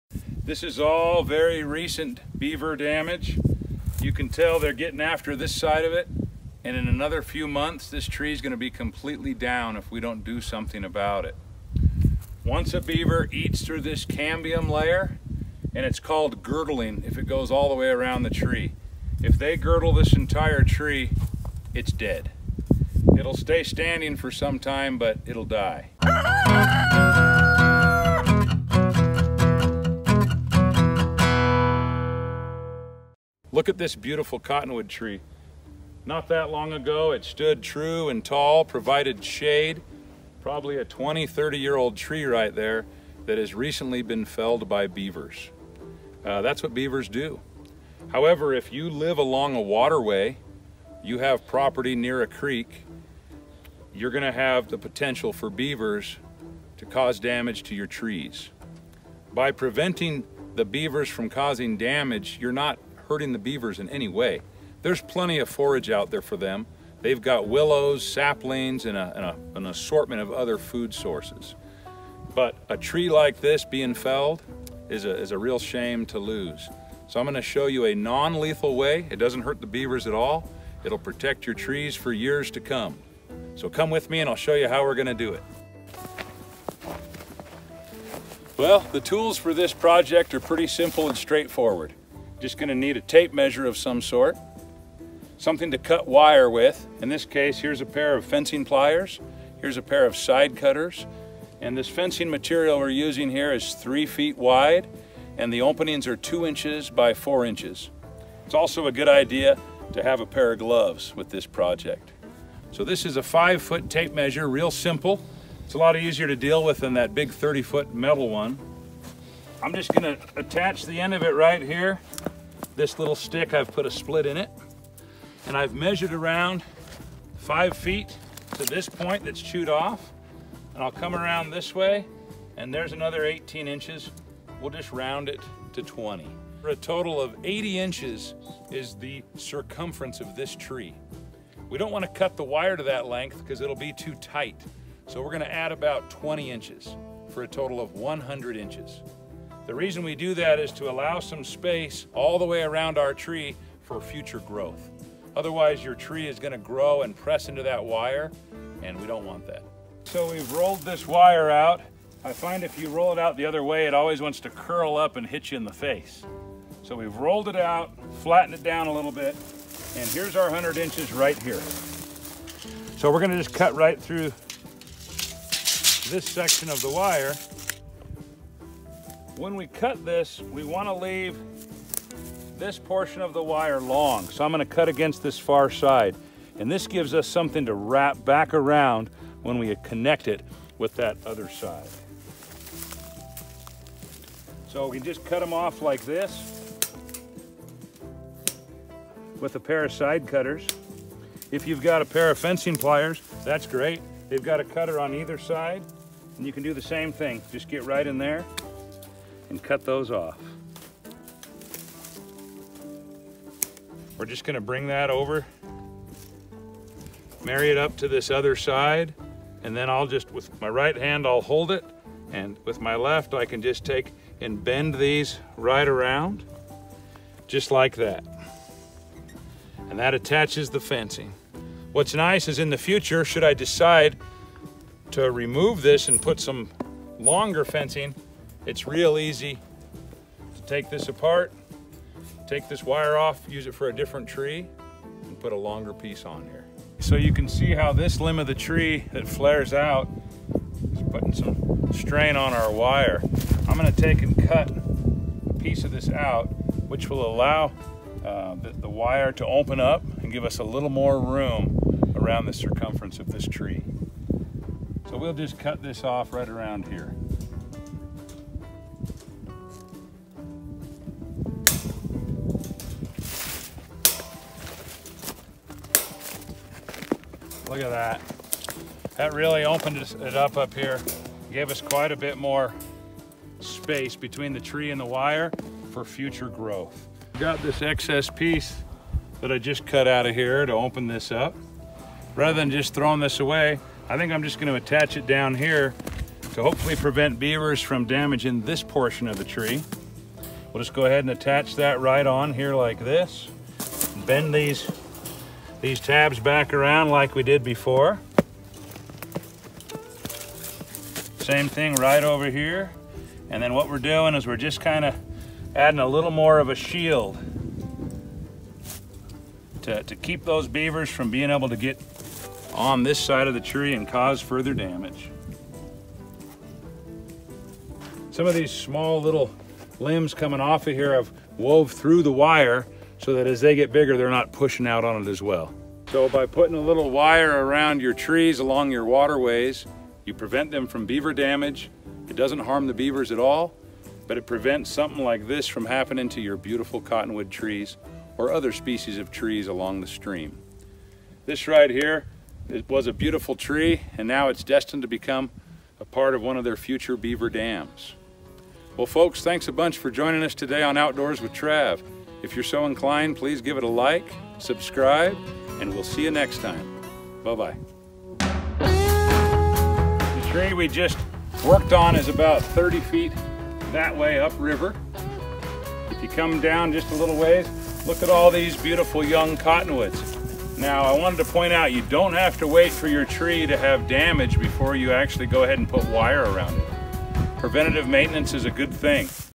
Keep Beavers Away from Trees – Outdoors with Trav
An easy and non-harmful way to keep beavers away from your trees. Trav shows how to install beaver fencing around trees to prevent beavers from destroying the trees you want to keep.

Outdoors with Trav brings you expert knowledge on making and doing things related to the outdoors.

To contact Trav email him at outdoorswithtrav [at] gmail [dot] com